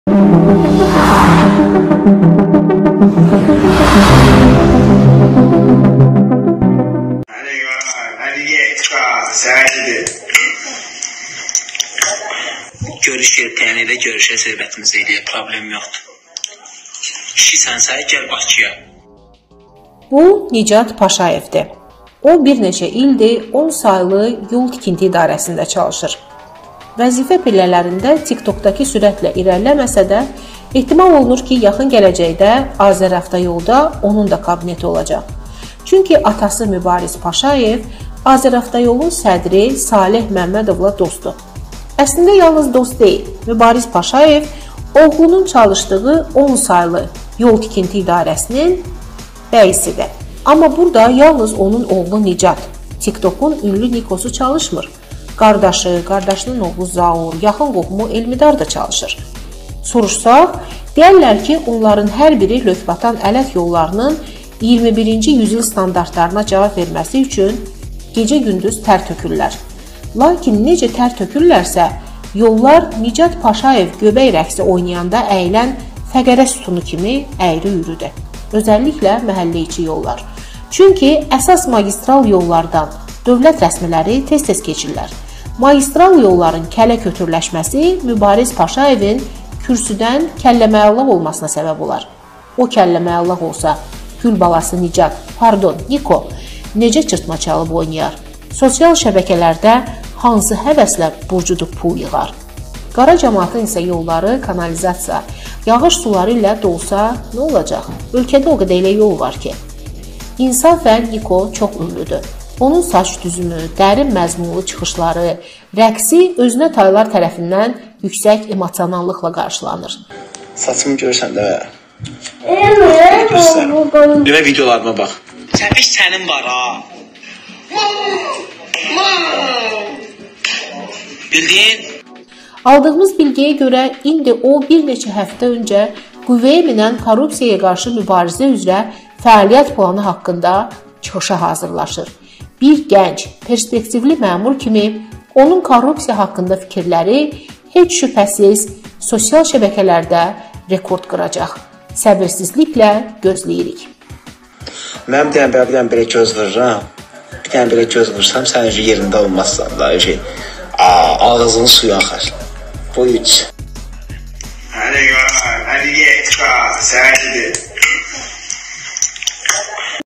10 saylı Yol İstismar MMC-nin direktoru Mübariz Paşayevin oğlunun “Tik-tok” MƏCARALARI
Papanın gül balası: 10 saylı Yol İstismar MMC-nin direktoru Mübariz Paşayevin oğlunun  “Tik-tok” MƏCARALARI